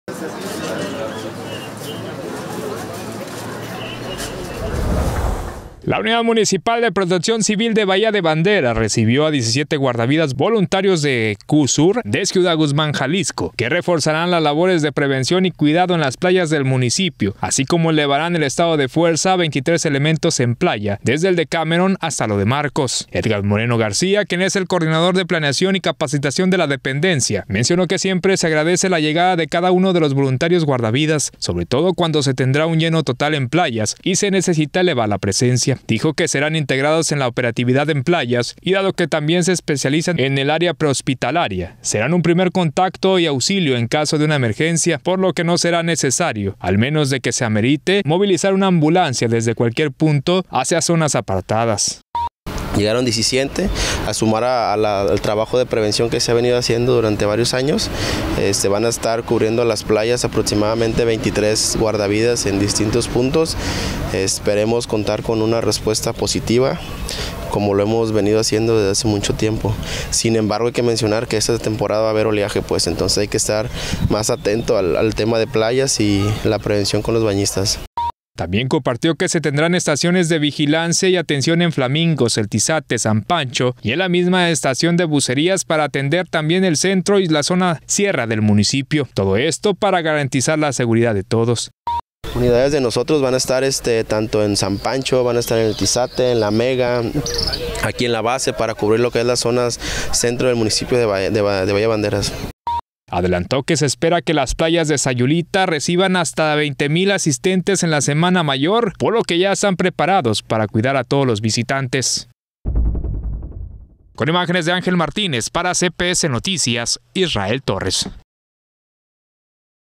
Llegan voluntarios a PC y Bomberos de Bahía para Semana Santa y Pascua |CPS Noticias Puerto Vallarta
🛟🌊 La unidad municipal de Protección Civil recibió guardavidas voluntarios de Ciudad Guzmán para reforzar labores de prevención esta Semana Santa.